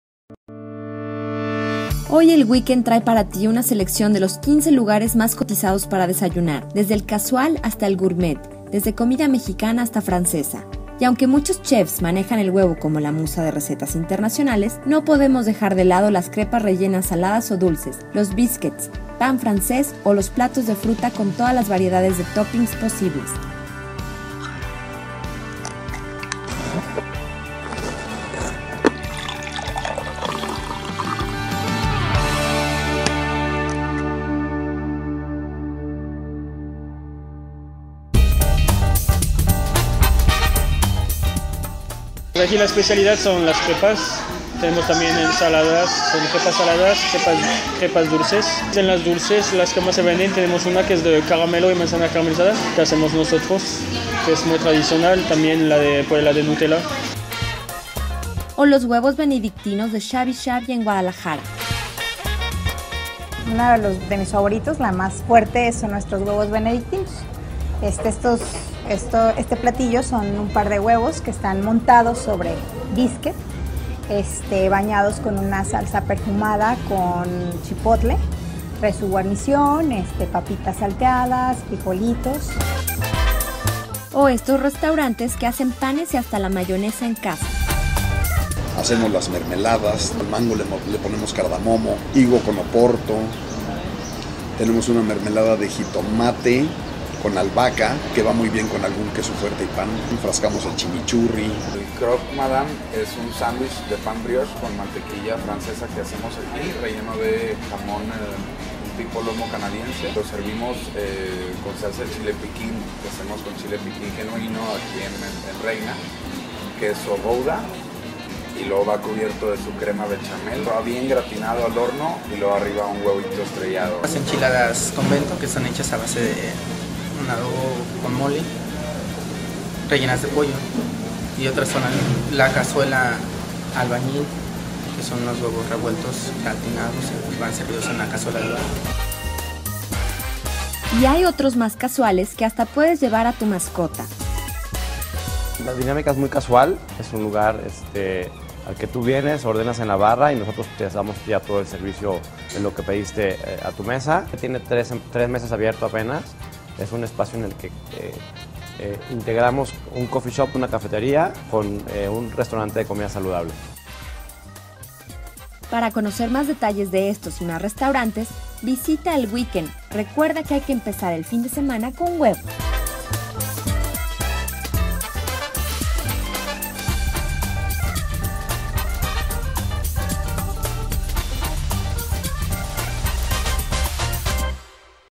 El Wiken 110: ¡Buenos días! ¿Qué se te antoja para desayunar?
Hoy El Wiken trae para ti una selección de los 15 lugares más solicitados para empezar el día.